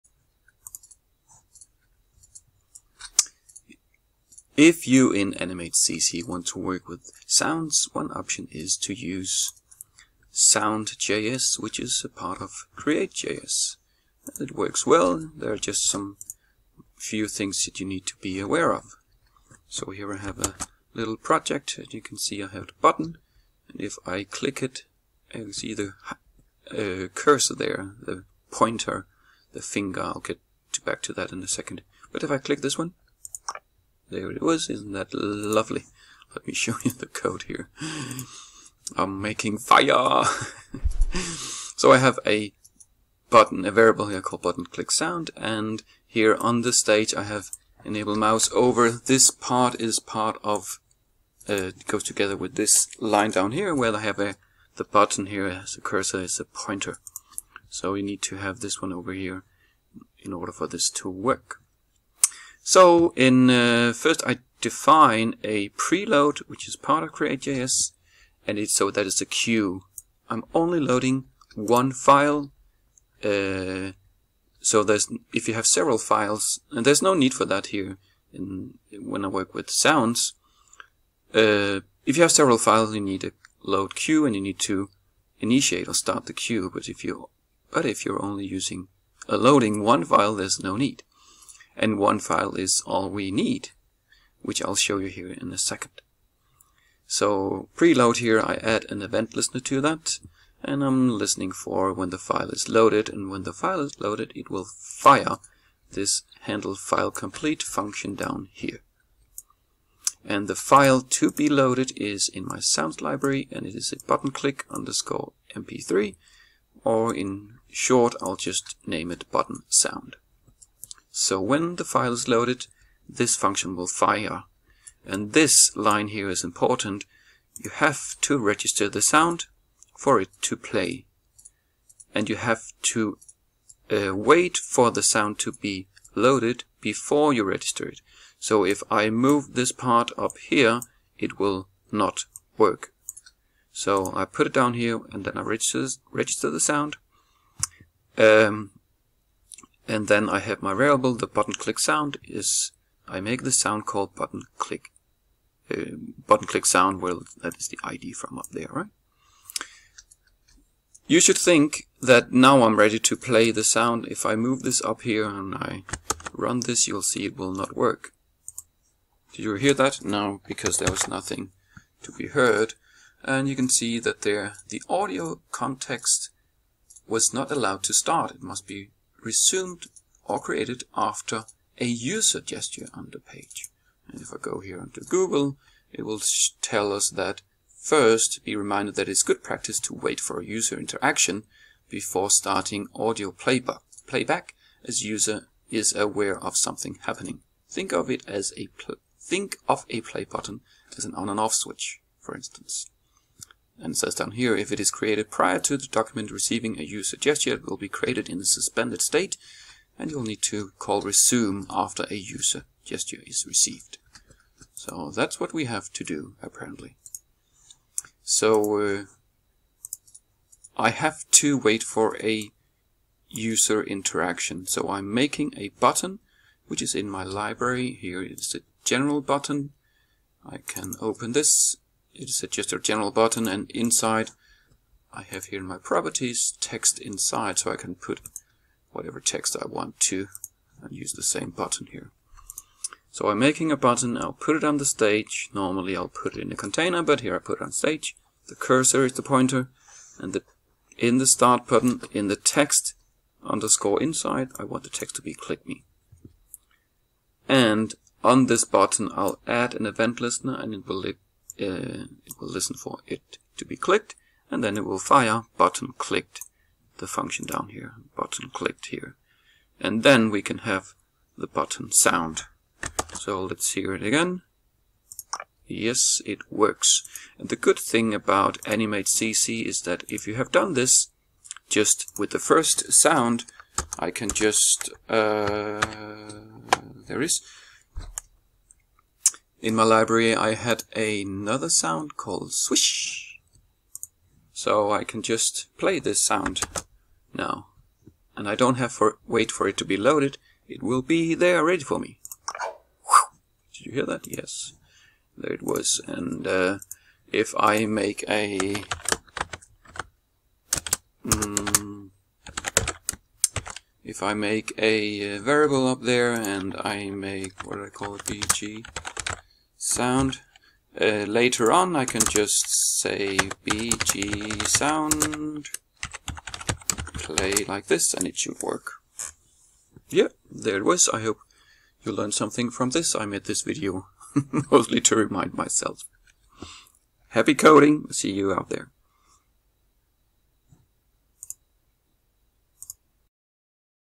SoundJS and Animate CC (CANVAS)
Short intro on how to use soundjs in Animate CC (html5 Canvas)